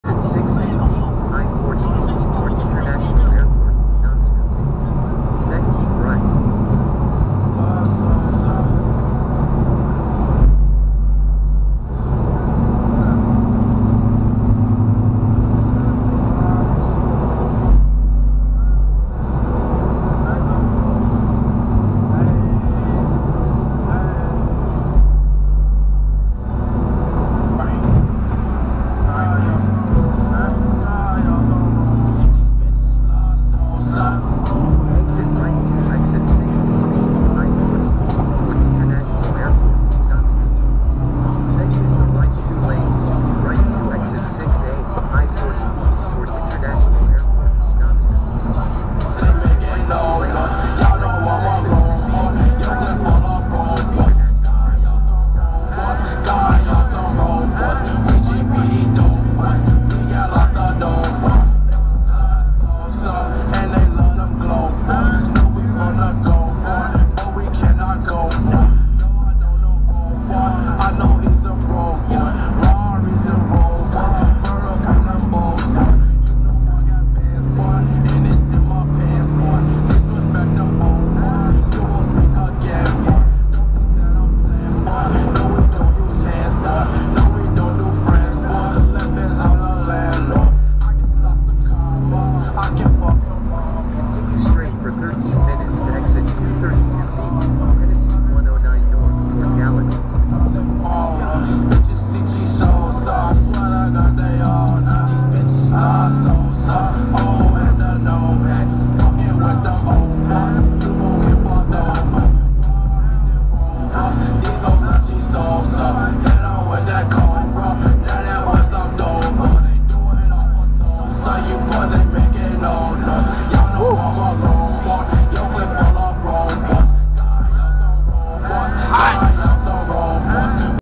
No Hesi
NPC signals right, but merges left. Then the van and suv both try to take the lane at the same time.